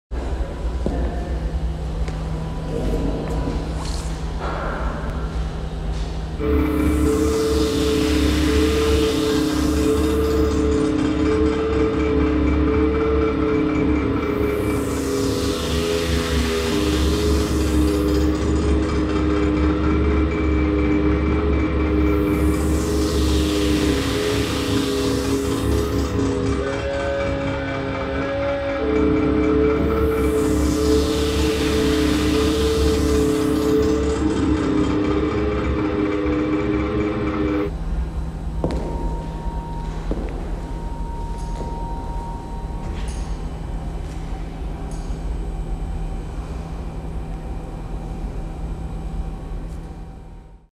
Kevin Beasley with Taja Cheek | Performance from Kevin Beasley: A view of a landscape
Artist Kevin Beasley and multi-instrumentalist, songwriter, and vocalist Taja Cheek perform a sound composition as part of the exhibition "Kevin Beasley: A view of a landscape." Taking the noise generated from a running cotton gin motor, Cheek and Beasley manipulate and distort the sound into an immersive aural experience.

This is the first of four performances in conjunction with "Kevin Beasley: A view of a landscape." The exhibition is on view through March 10, 2019. Learn more about the exhibition and buy tickets